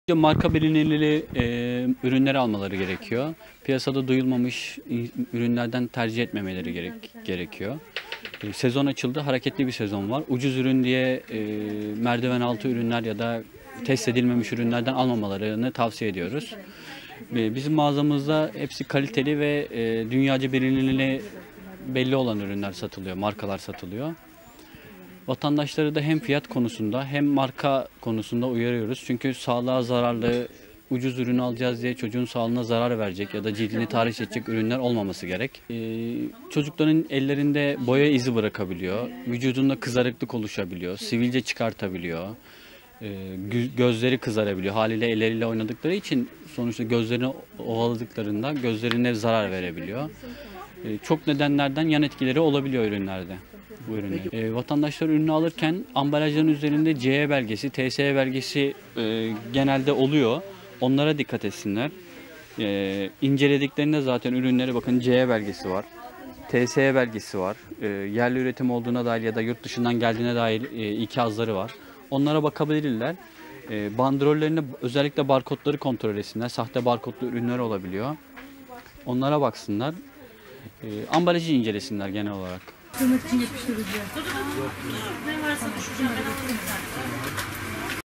Kırtasiyecilerden velilere uyarı
Yeni eğitim ve öğretim dönemi öncesi kırtasiyecilerde hareketlilik başladı. Velileri uyaran kırtasiyeciler, merdiven altı ürünlerin tercih edilmemesi gerektiğini söyledi.

Yarın başlayacak eğitim ve öğretim yılı öncesinde öğrencileri okul heyecanı, aileleri ise çocuklarının ihtiyaçlarının karşılama telaşı sardı. Kırtasiyeci Osman Durdu, velileri merdiven altı kırtasiye ürünlerine karşı uyardı. Velilerin bilinir markaları tercih etmeleri gerektiğini belirten Durdu, "Piyasada adı duyulmamış markaları almamaları gerekiyor. Ucuz olan ve 'merdiven altı' diye tabir edilen ürünler var. Bunlardan alınmaması gerekiyor. Bizim mağazamızda kaliteli ve dünyaca bilinirliliği olan ürünler satılıyor. Vatandaşları hem fiyat konusunda hem marka konusunda uyarıyoruz" dedi.

"CE ve TSE belgeleri kontrol edilmeli"

Çocukların ellerinde boya izinin kalabileceğini, kaşınma ve yanmaların olabileceğini belirten Durdu, "Çocukların ellerinde boya izi bırakabiliyor. Vücutta kızarıklık oluşabiliyor, sivilce çıkarabiliyor, gözleri kızarabiliyor. Çok sebeplerden yan etkileri olabiliyor. Vatandaşlar ürünü alırken ambalaj üzerinde CE ve TSE belgesi olup olmadığını kontrol etmeleri gerekiyor. İncelediklerinde bunları görebilirler. Bandrolleri ve barkodları kontrol edebilirler. Sahte barkotlu ürünler olabiliyor. Genel olarak ambalajı incelemelerini tavsiye ederim" şeklinde konuştu.